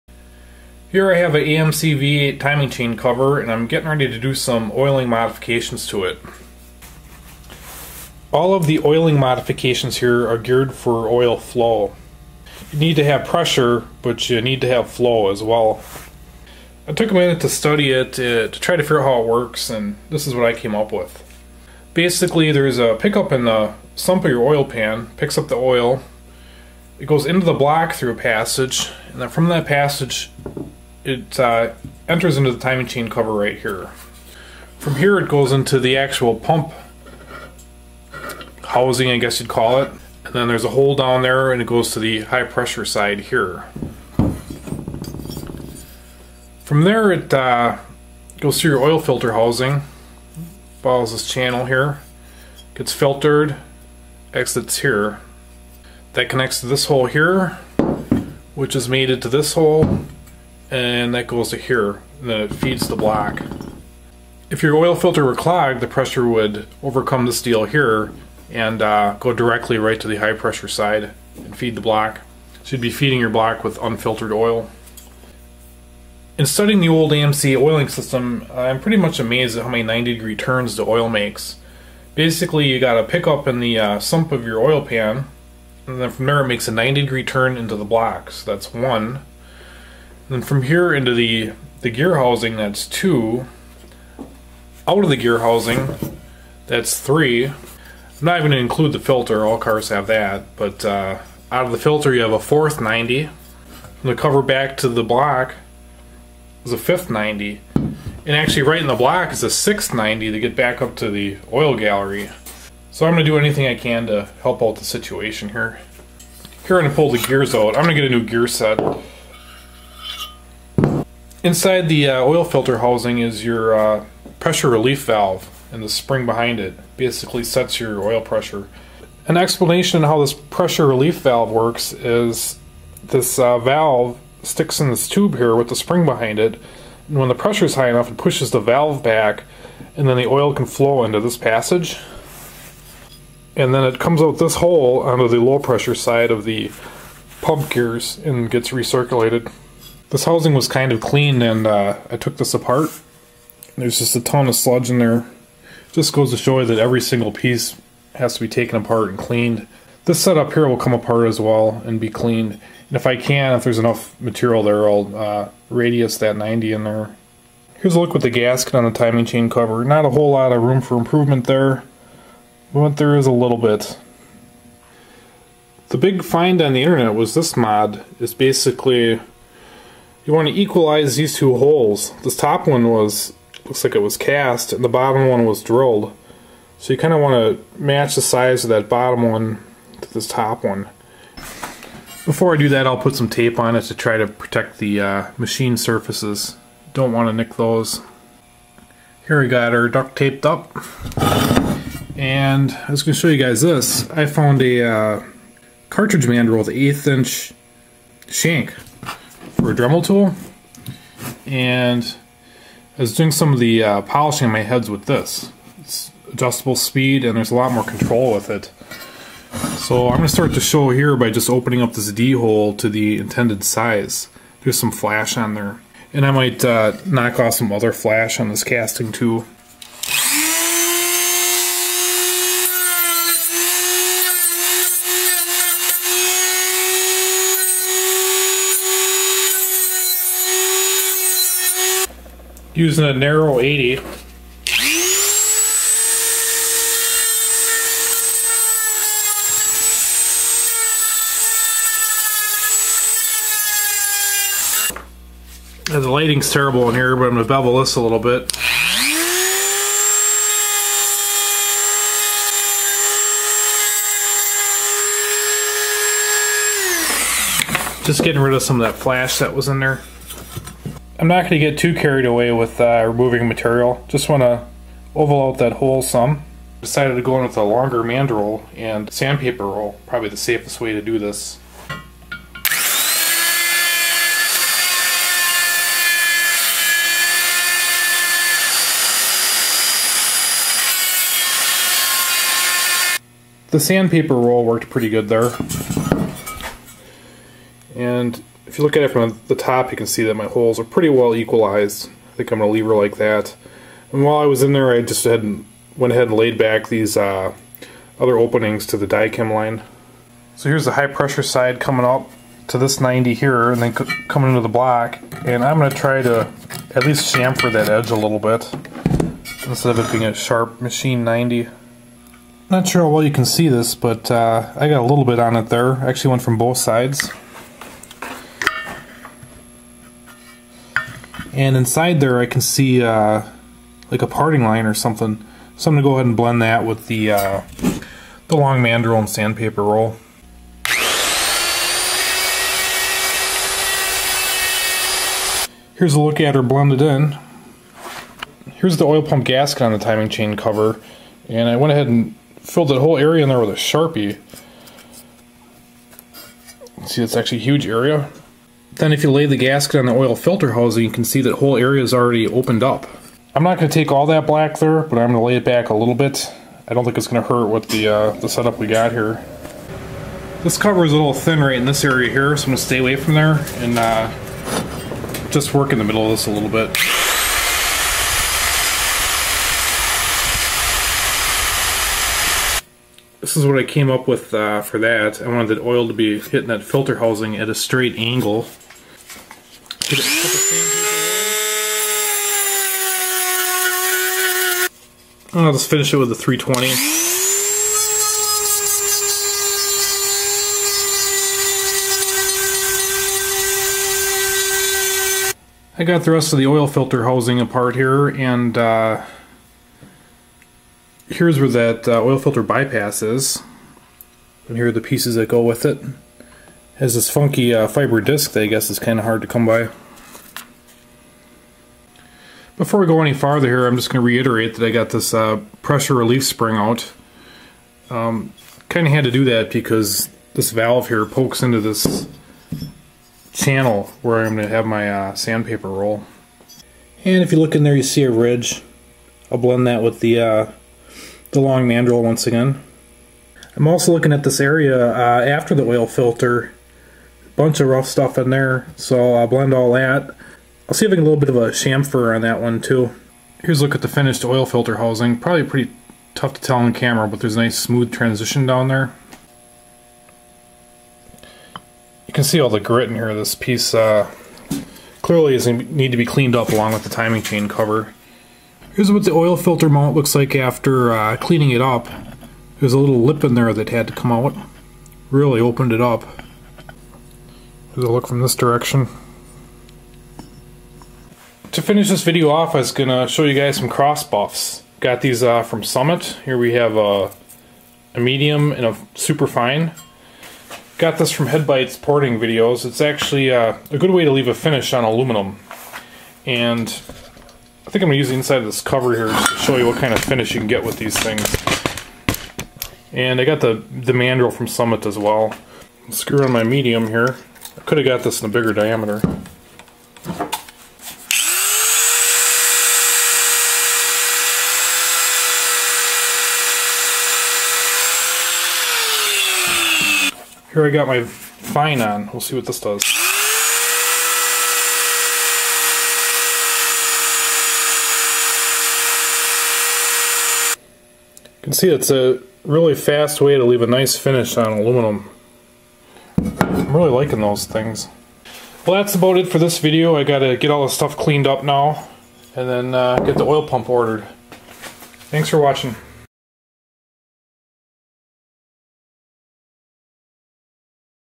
AMC Timing Cover Oiling Modifications V8
Trying to get the oil to flow in the old AMC 401.